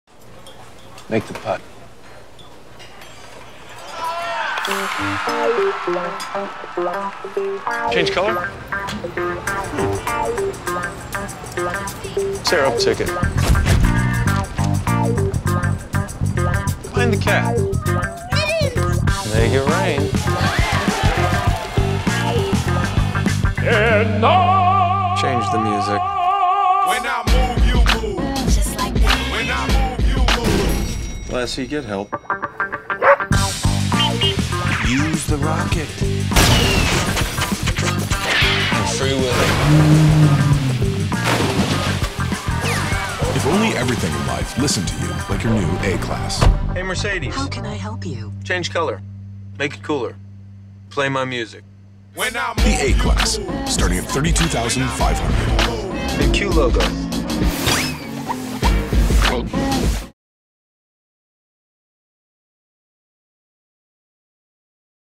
Mercedes Benz 2019 A-Class sedan commercial
Η νέα διαφήμιση της Mercedes-Benz A-Class sedan που θα προβληθεί τη μέρα του Super Bowl στο Mercedes-Benz stadium της Ατλάντα είναι απλά απολαυστική, βάζοντας στο επίκεντρο το νέο σύστημα MBUX και όχι μόνο.